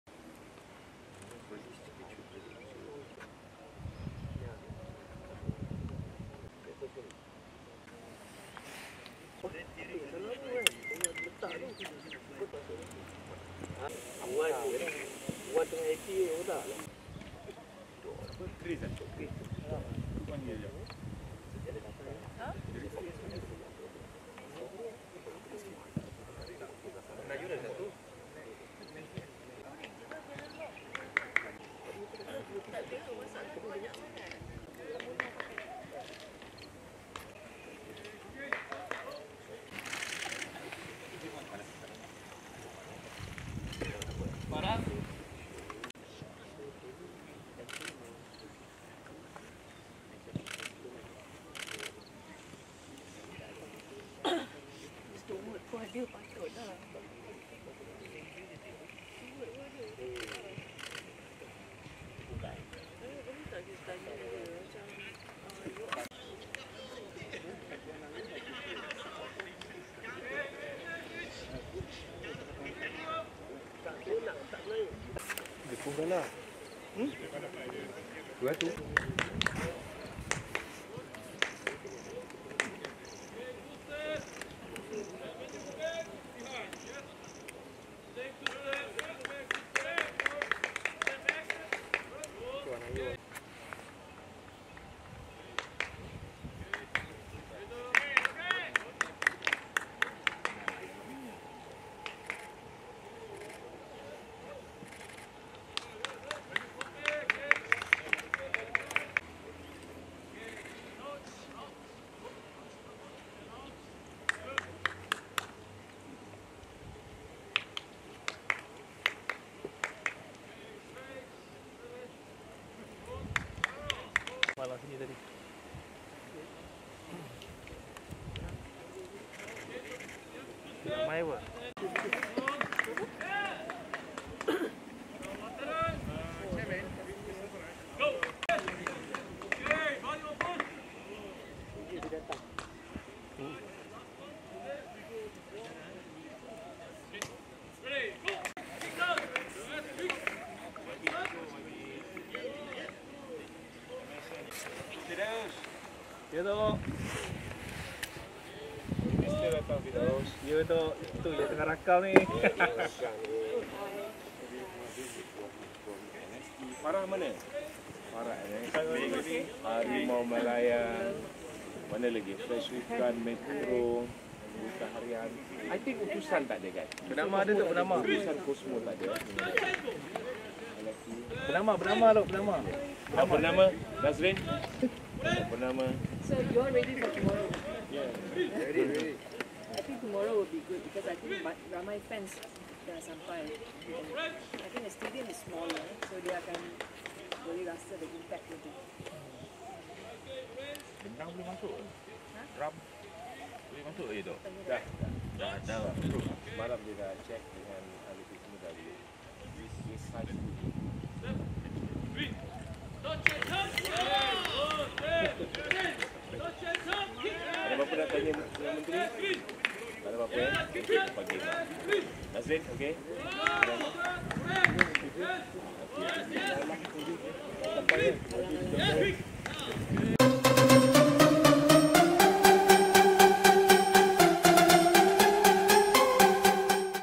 Extended Training Hari ke-17 | YB Hannah Yeoh turun padang ke Doha - demi Harimau Malaya!
Rakaman sesi latihan skuad Harimau Malaya menjelang perlawanan kedua kumpulan E menentang pasukan Bahrain pada 20 Januari di Jassim bin Hamad Stadium, Al Rayyan.